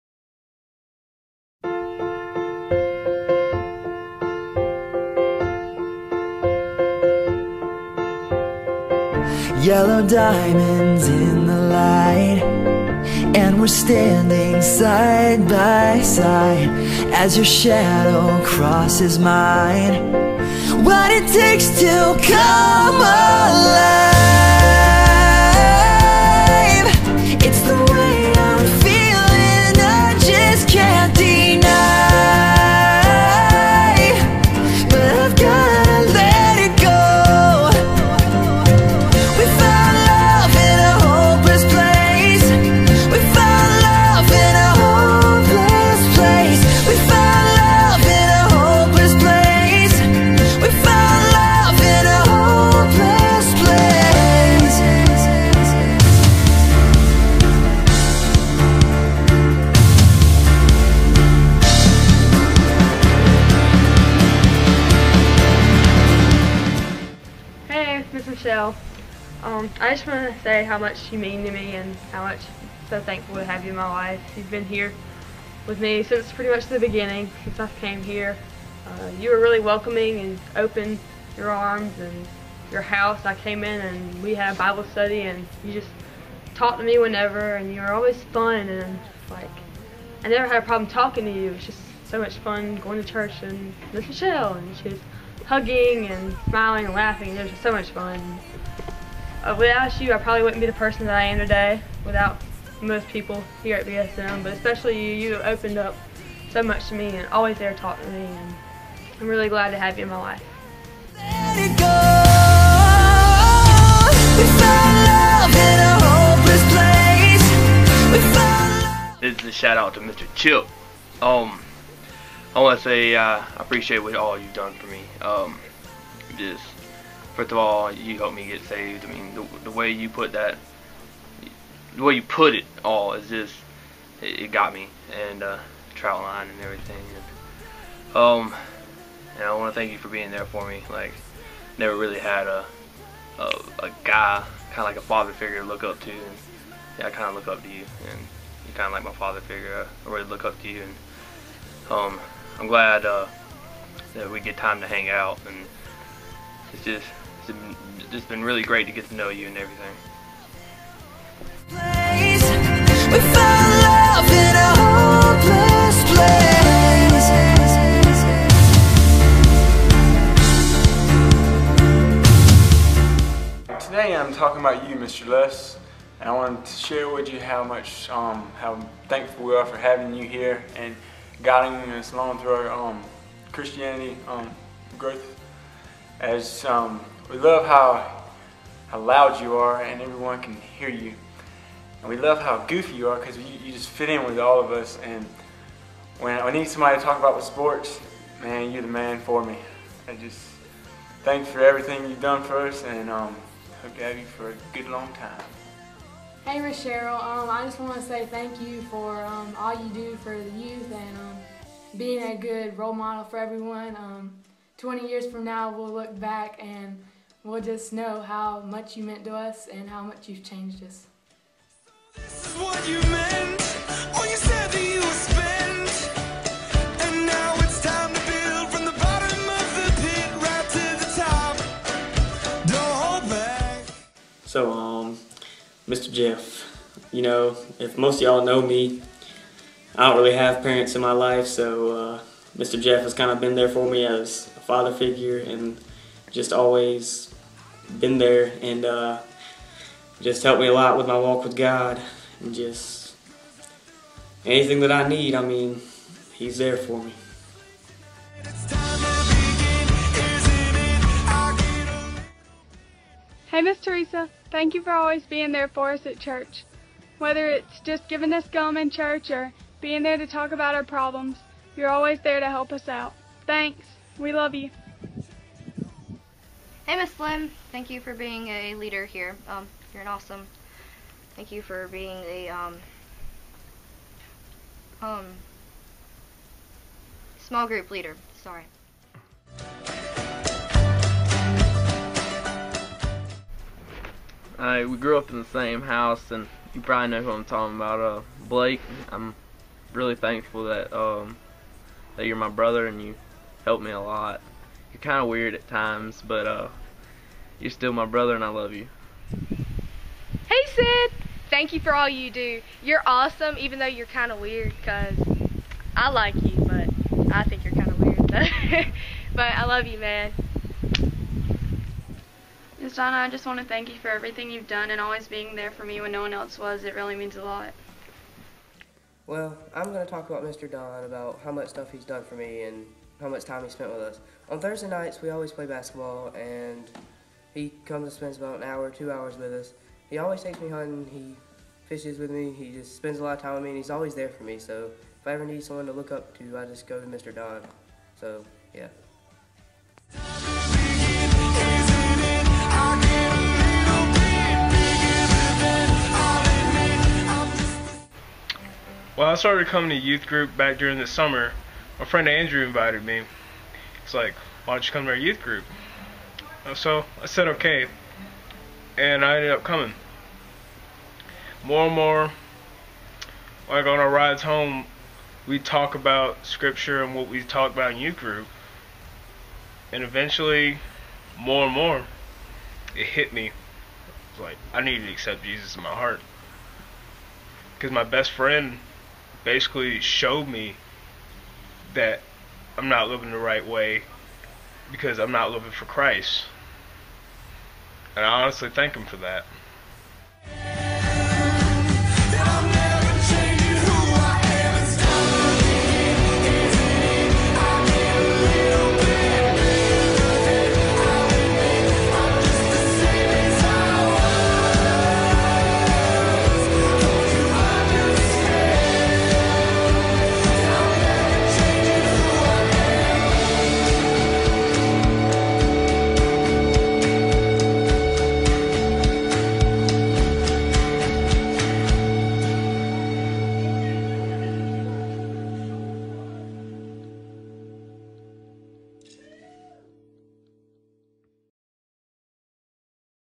Thank You BSM Leaders!
Words can't express how much it means to have people who care about students they way ours do out at Bethel Student Ministries. We are blessed. We are honored. Thank you for serving Jesus by serving others!